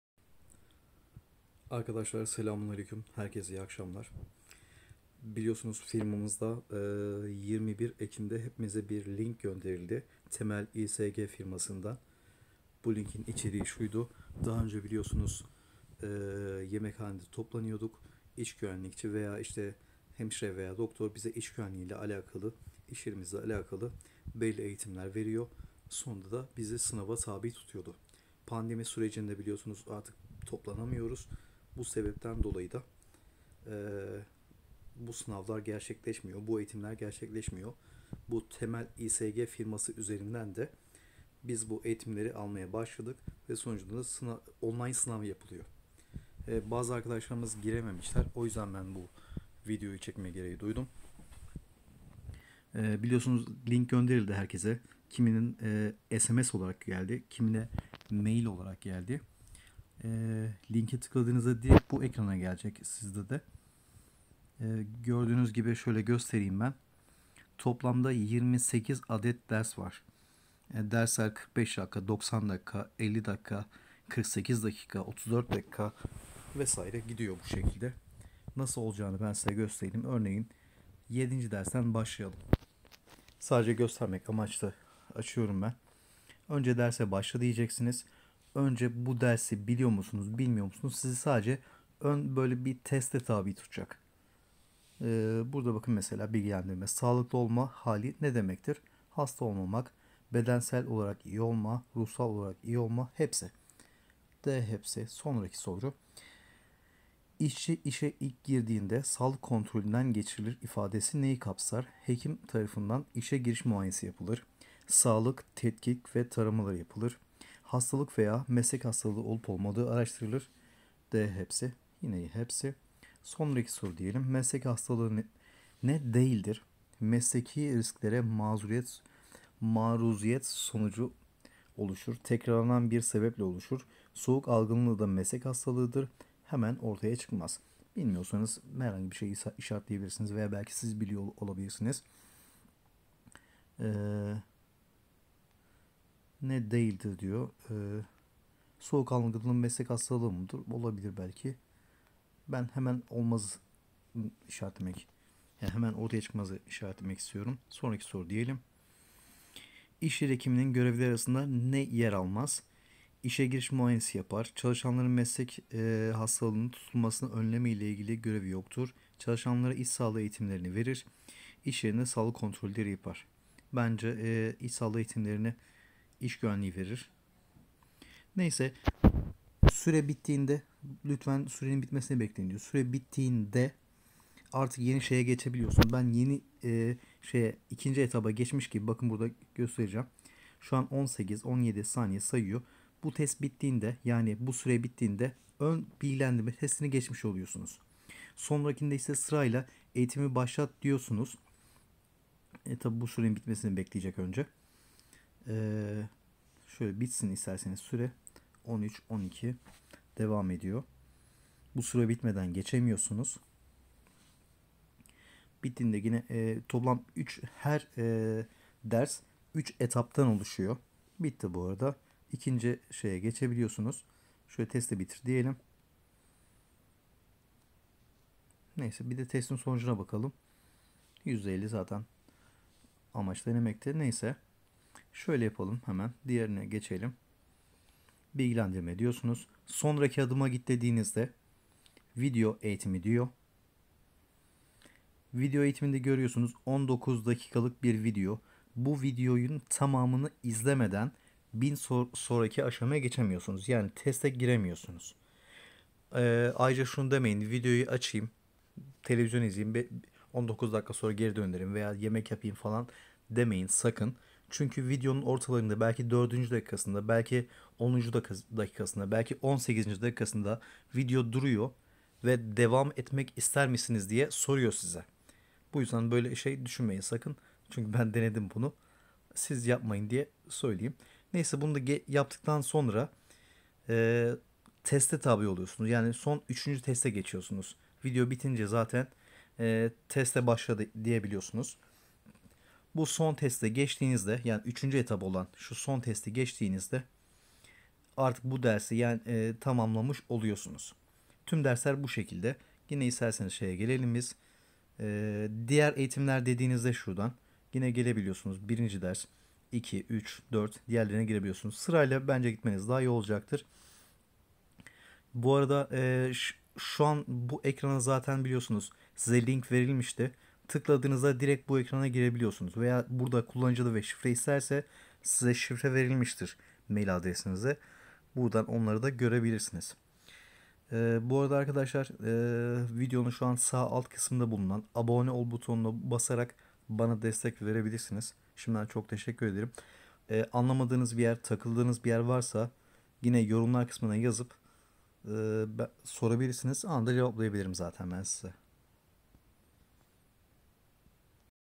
temel isg eğtim.
aslar pres bektaş firması çalışanlarına özel isg egtimleri